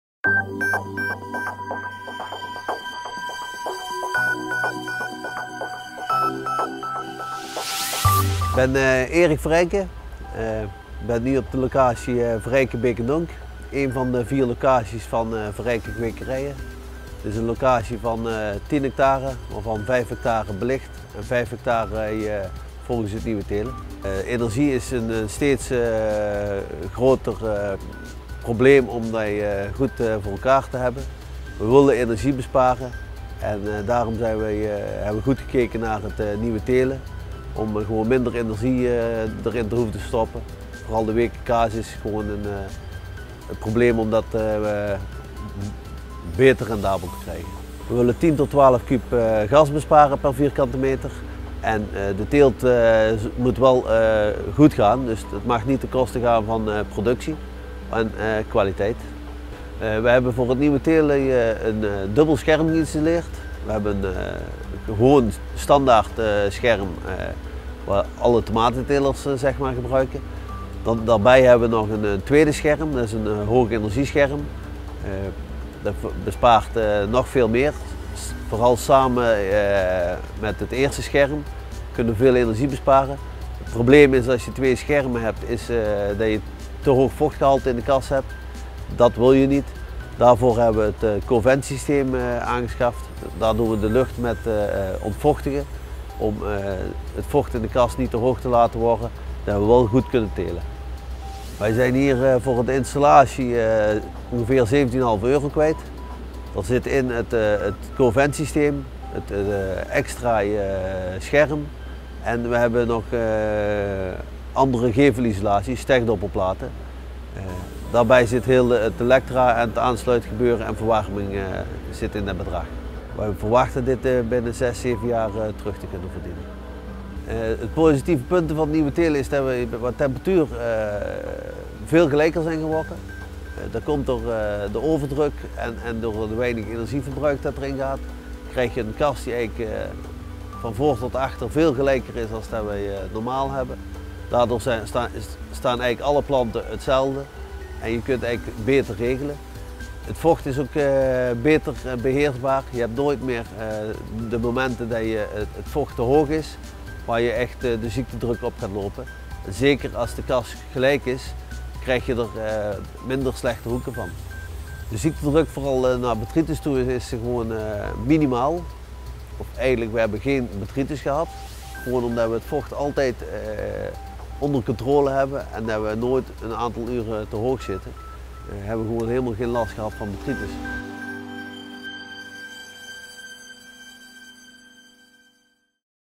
Nieuwe Telen Eric Vereijken
Tomatenkweker Eric Vereijken vertelt over zijn ervaringen en behaalde resultaten met Het Nieuwe Telen voor de Innovatie Arena. De Innovatie Arena's georganiseerd door Greenport Campus brengen thema's met breed belang onder de aandacht in de glastuinbouw. Kwekers, leveranciers en andere betrokkene uit de branche worden uitgenodigd om verschillende statische presentaties te bekijken en hierbij vragen te stellen. Meer informatie over de Innovatie Arena's vindt u op